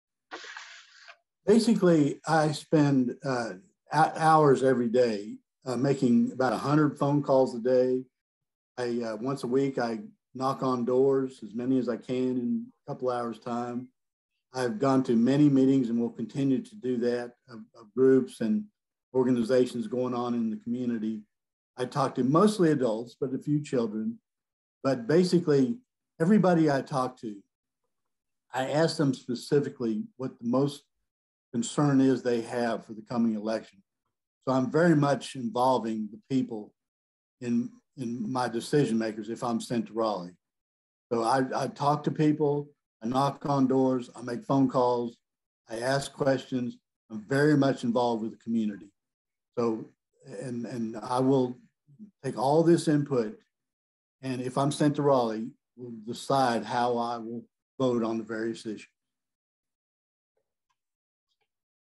Alvin Reed: How do you respond to public opinion and involve youth in your decision making?
Alvin Reed, candidate for NC State Senate district 20.